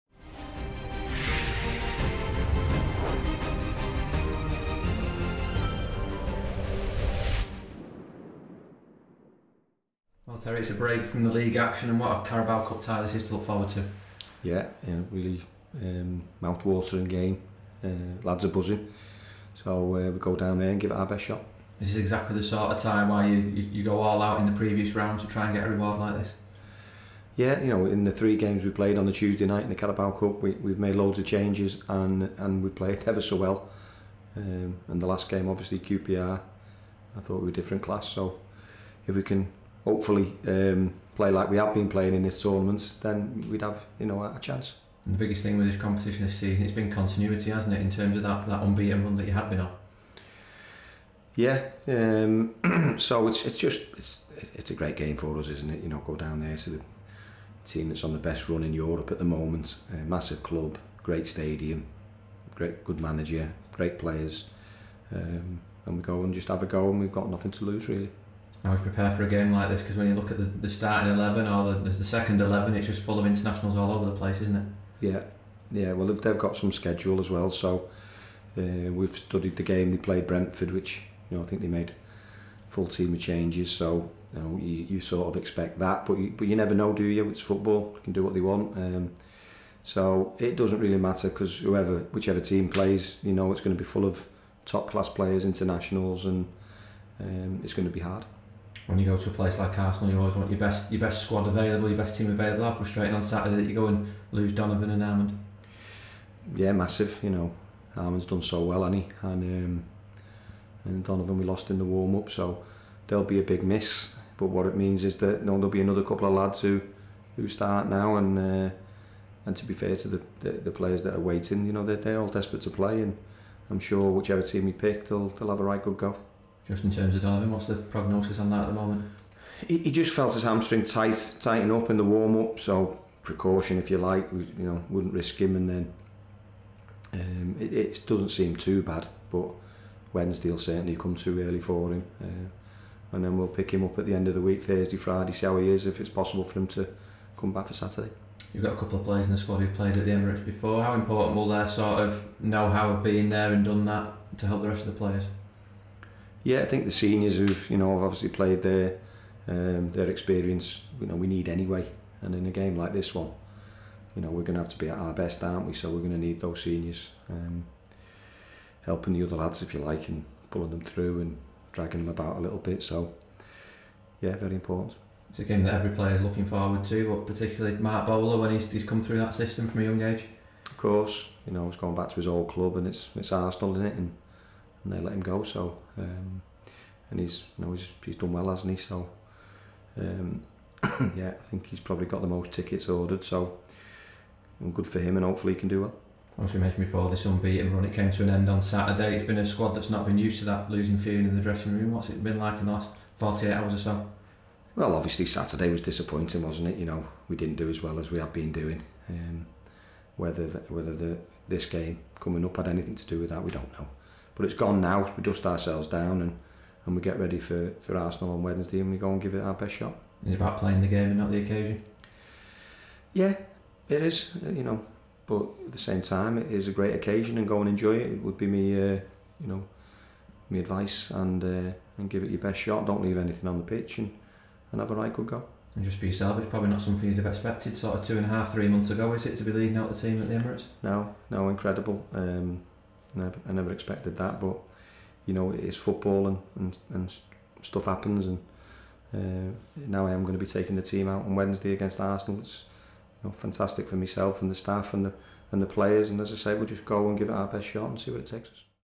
Arsenal Preview | Terry McPhillips
Manager Terry McPhillips gives his thoughts ahead of Blackpool's fourth round Carabao Cup tie at Arsenal on Wednesday.